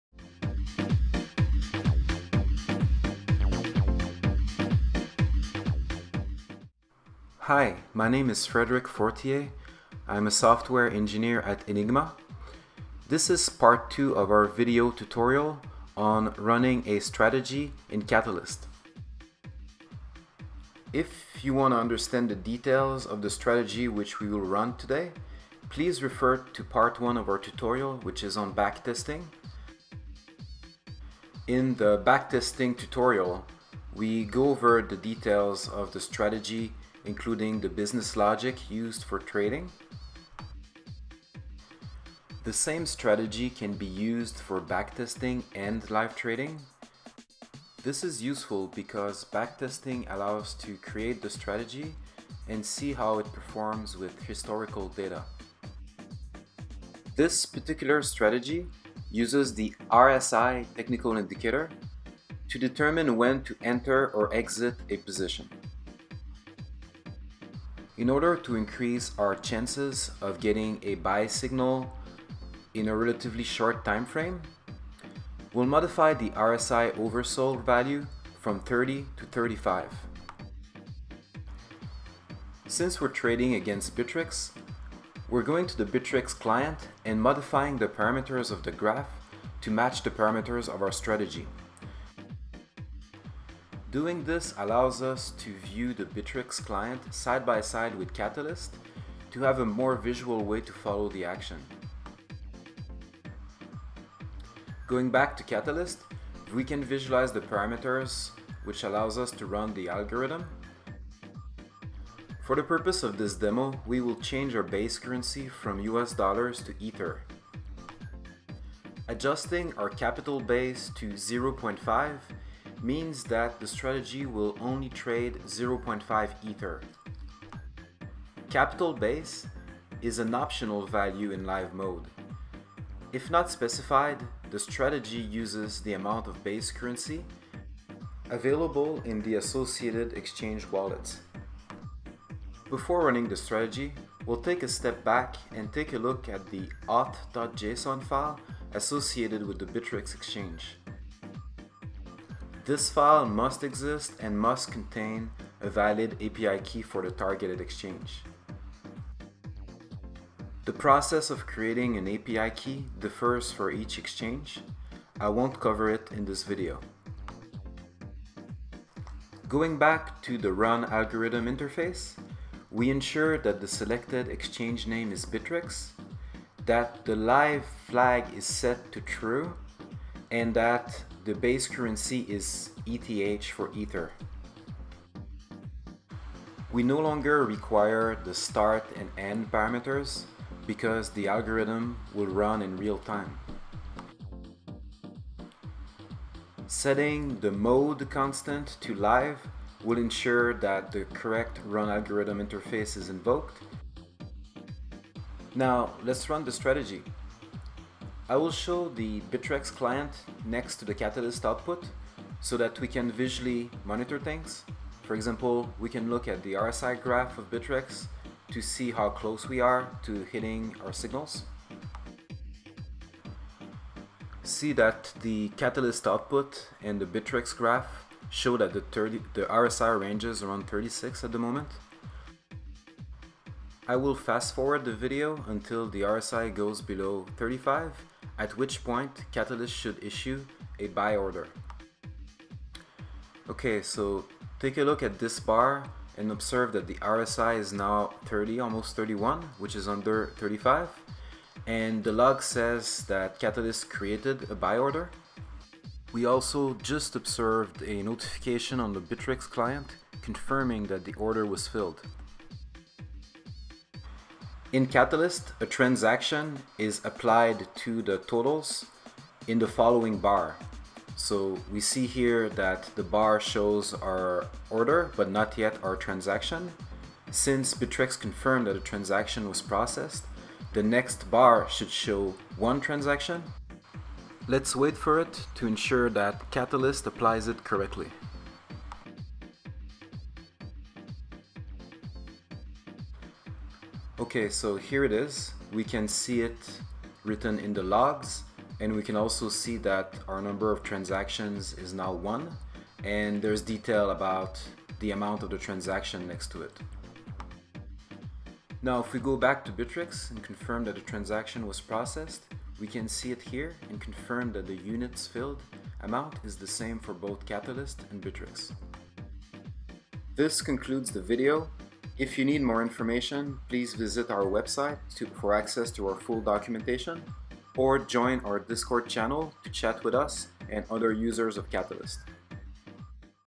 Catalyst - Live Trading a Strategy
Welcome to Enigma | Catalyst - our platform for data-driven crypto investment and trading.

This video covers how to trade live an algorithm (based on a mean reversion strategy) in Catalyst, as documented in This is the second part of this other video where we started with the very same algorithm in backtesting mode.

Please leave feedback in the comments below!